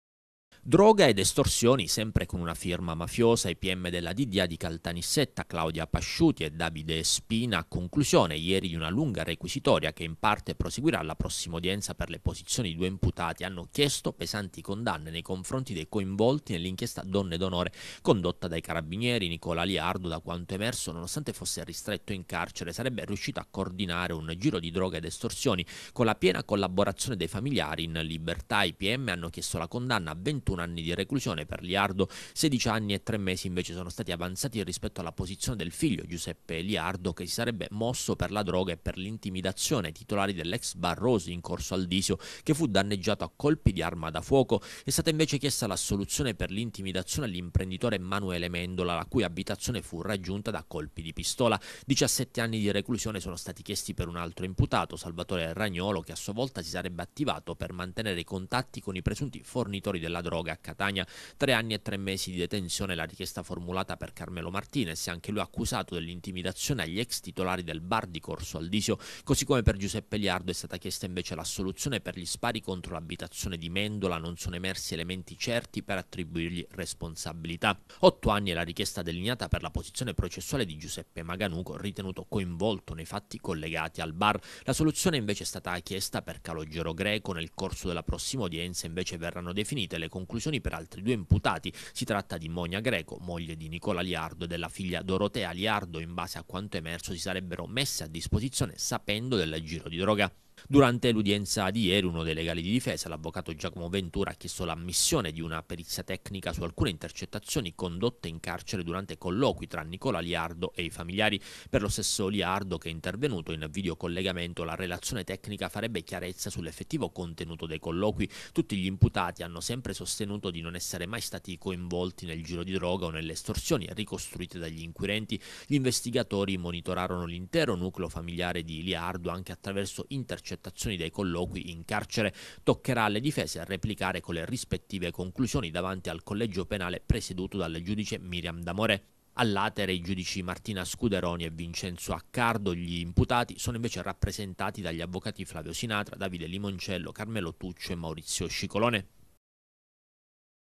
Gela, droga ed estorsioni: chieste pesanti condanne
L’inchiesta denominata “Donne D’onore”, ha fatto luce su diversi episodi di spaccio ed una intimidazione ad un imprenditore